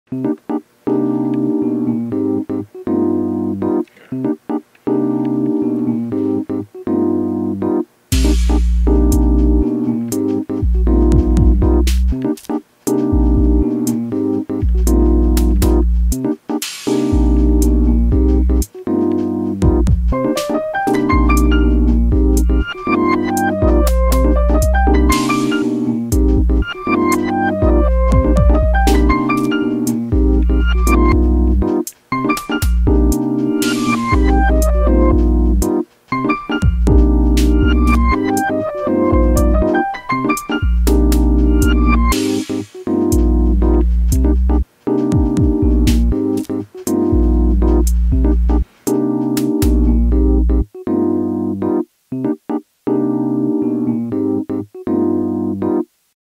8k footage
very old camera test of me not knowing how to color grade r5 8k footage that is now taking up 200 gigs on my hard drive